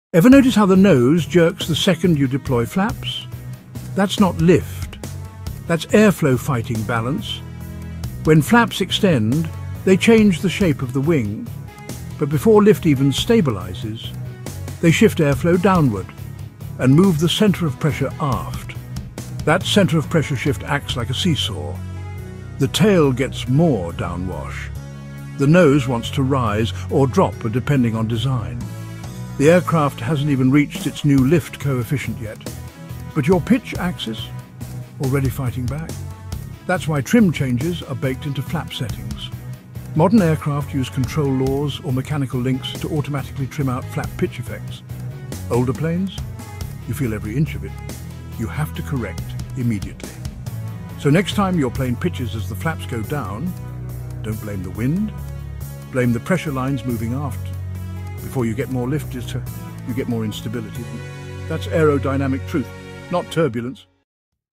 The Real Reason Planes Pitch on Flap Extension
Why does the nose pitch the second you drop the flaps—before lift even kicks in?
In this crisp explainer, we dig into the real reason aircraft pitch instantly when flaps extend… and it’s not what most think.

📉 It’s not the added lift.
💨 It’s the shifting airflow.
Flap extension moves the center of pressure, alters downwash, and sends signals to the tailplane faster than your airspeed reacts. The result? Nose-up (or down) moments that pilots feel immediately.

We break down:
– Why some aircraft pitch up, others pitch down
– How flight control computers fight back
– And how you’re reacting to pressure long before performance

🎥 Watch now to decode the flap-pitch connection every pilot should understand.